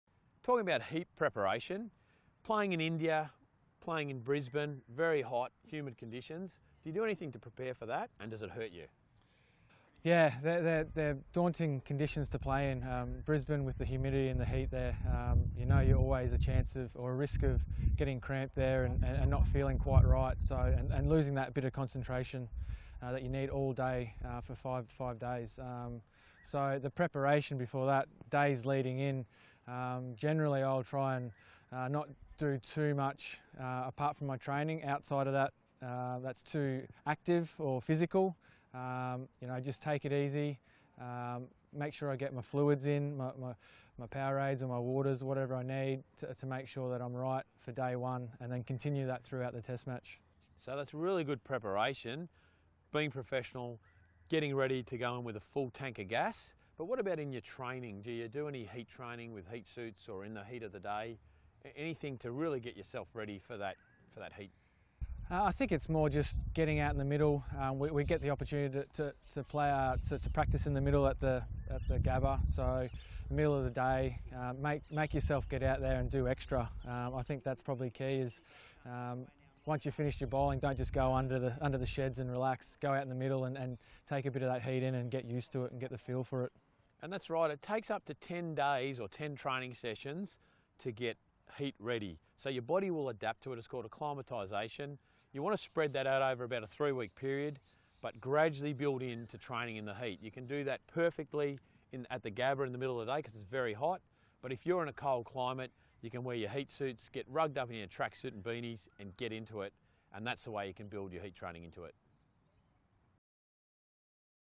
BOWLFIT Masterclass with Mitchell Johnson: Heat Preparation
Dec' 2018: With temperatures set to soar during the Aus v Ind Test Series in Australia, Jock & Mitch tell explain what  you can do to prepare for these conditions and still perform at your best.